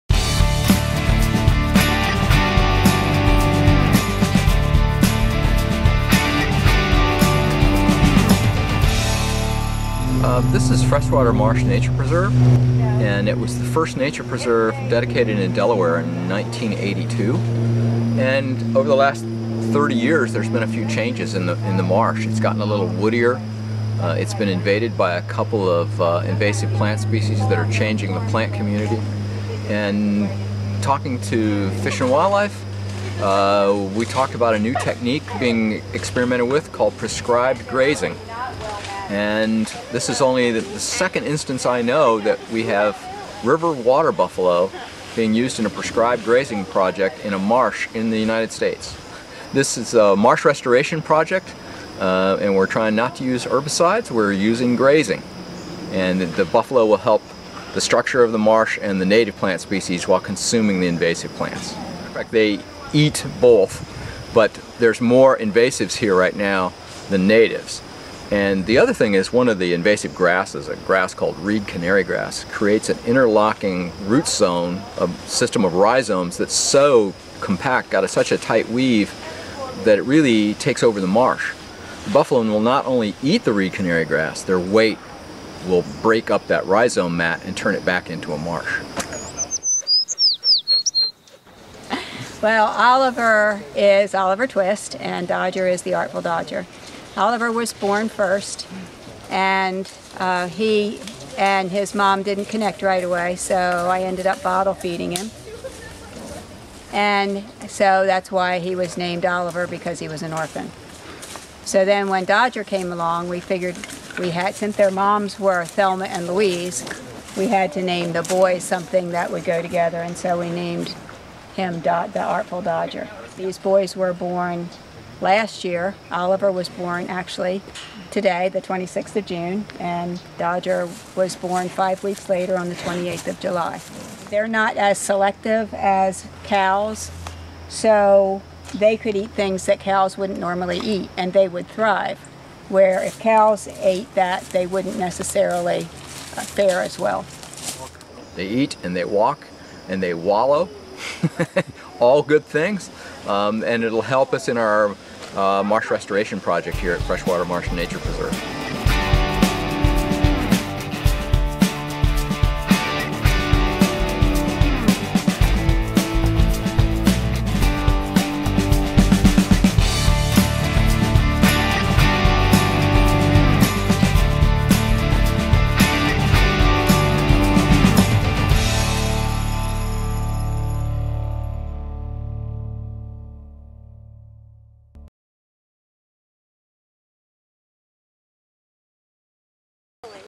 Your DNREC - Water Buffalo
There's Water Buffalo in the Marsh at Brandywine Creek State Park! Find out why...